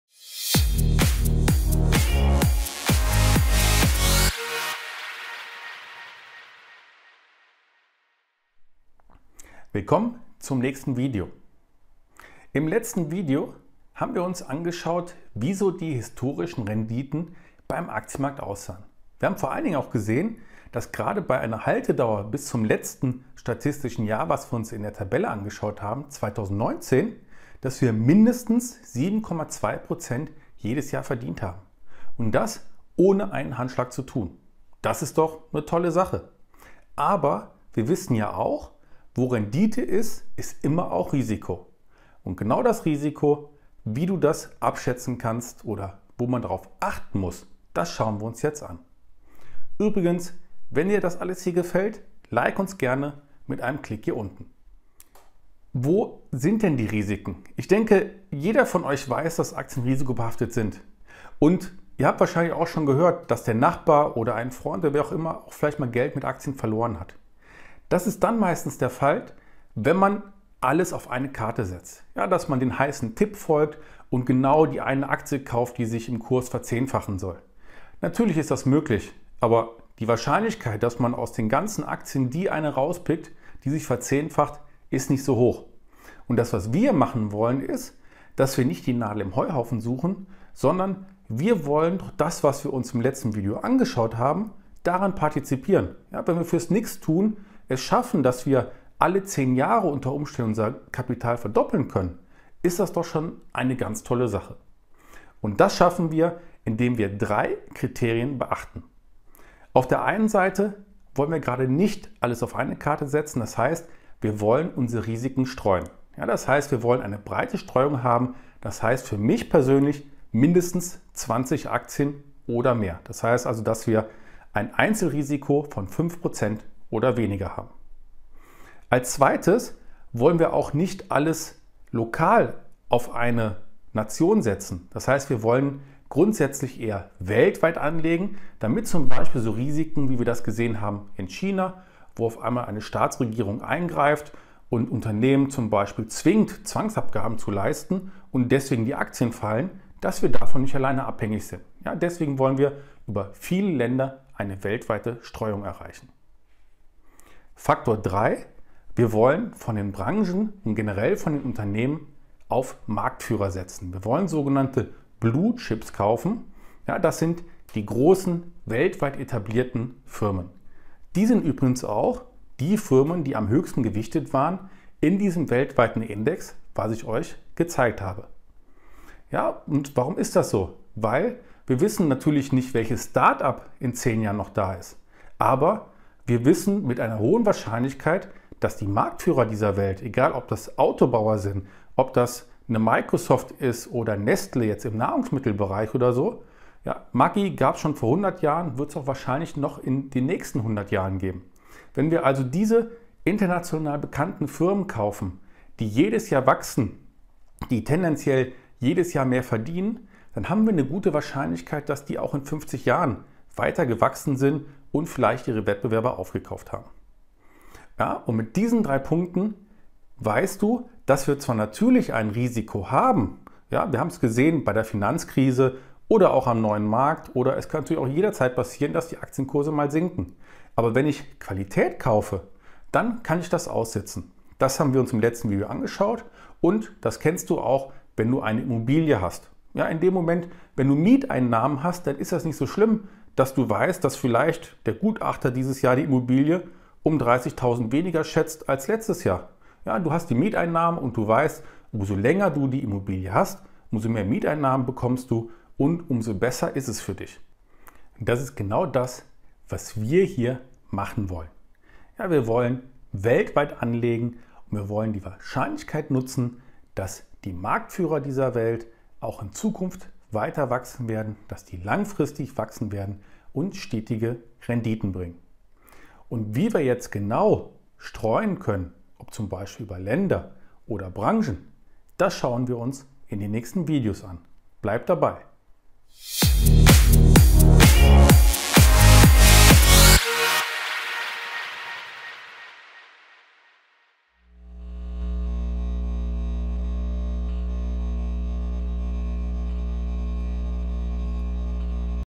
Aktienhandel ohne Risiko? 3 einfache Regeln
Aktienhandel ohne Risiko geht das? Mit 3 einfache Regeln lässt sich das Risiko reduzieren.
Der Handel mit Aktien ist immer mit einem Risiko verbunden. Ganz vermeiden lässt es sich leider nicht. Es gibt aber Möglichkeiten das Risiko beim Aktienhandel zu reduzieren. Einfach die Aktien des nächstbesten Startups kaufen oder das ganze Kapital in den Aktientipp des Nachbarn stecken, ist sicher keine gute Idee. Marc zeigt im dritten Teil der Serie „Aktien richtig handeln“ welche 3 einfache Regeln man beim Portfoliomanagement beachten sollte, damit das eigene Aktienportfolio auf Dauer wertstabil bleibt.

Rendite ist immer mit Risiko verbunden. Daher ist es wichtig, dass wir lernen, wie wir das Risiko abschätzen können.

Dafür gibt es drei wichtige Kriterien, die wir beachten sollten: erstens wollen wir unser Risiko streuen, indem wir mindestens 20 Aktien oder mehr kaufen, zweitens sollten wir nicht alles auf ein Land setzen, sondern lieber weltweit anlegen und drittens wollen wir auf Marktführer setzen, also bekannte und etablierte Unternehmen (blue chips), die jedes Jahr wachsen.

Mit diesen drei Punkten haben wir eine gute Wahrscheinlichkeit, dass wir langfristig wachsen und stetige Renditen erzielen. Wie wir genau streuen, z.B. bei Ländern oder Branchen, schauen wir uns in den nächsten Videos an. Also bleib dran!